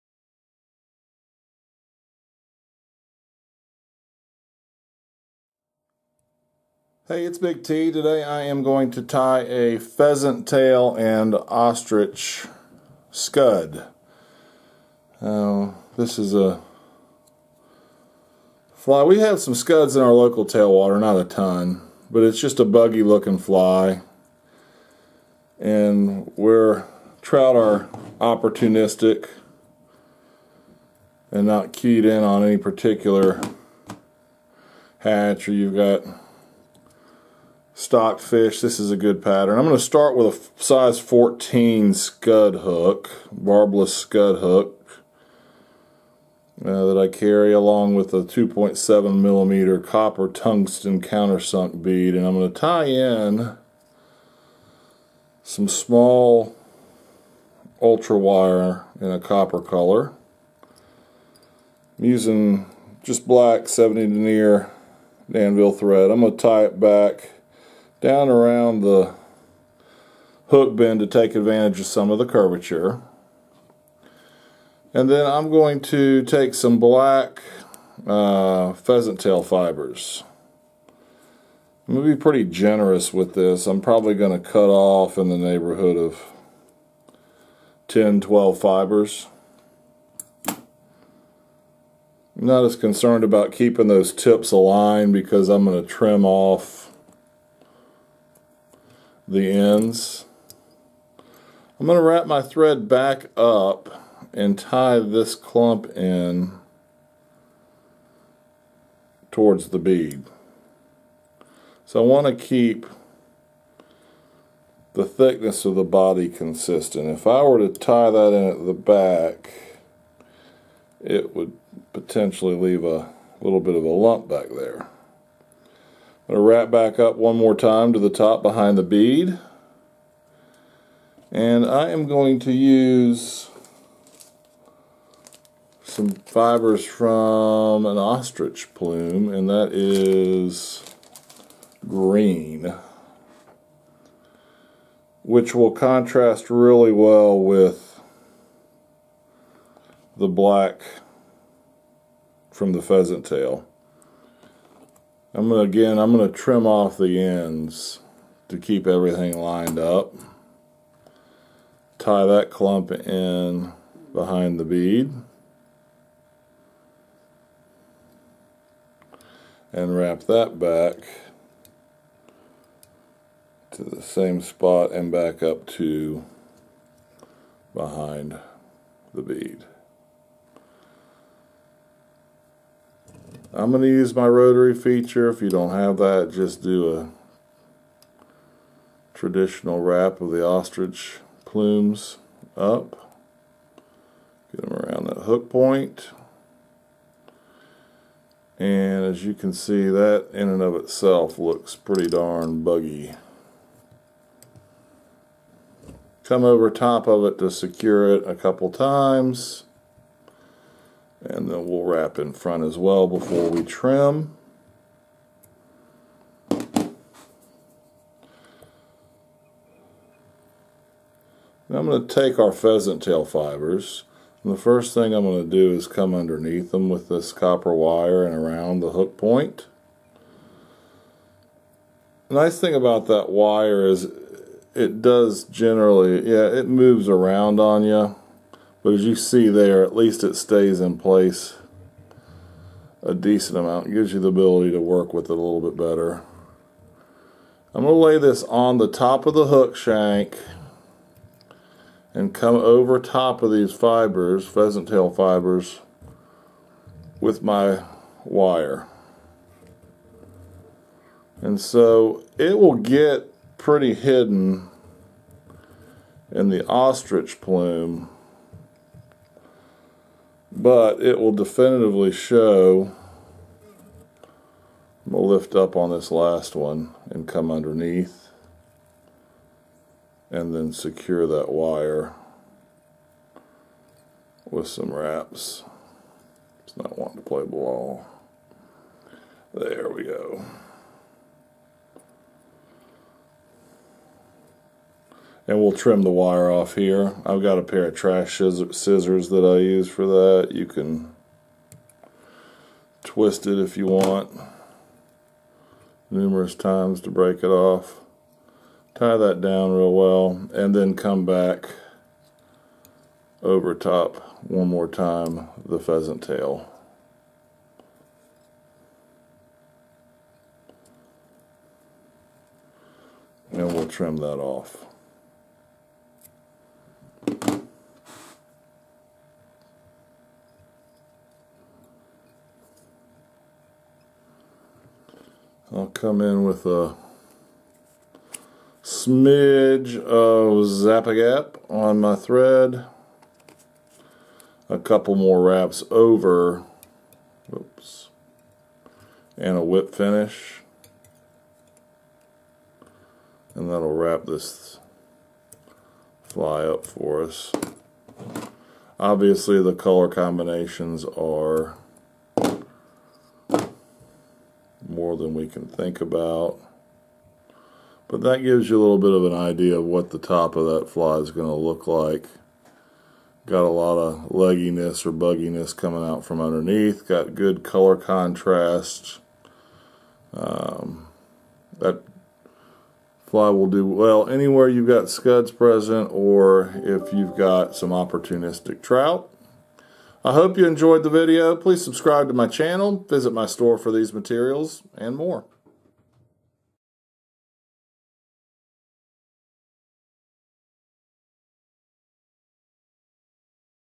PTO Scud
Pheasant tail and Ostrich plume make for a great scud .